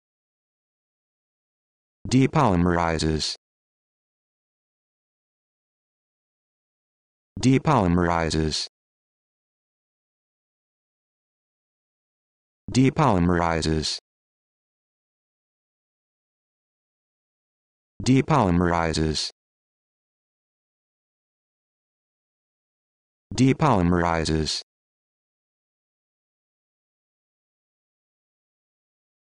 How to Pronounce Depolymerizes
This video shows you how to pronounce Depolymerizes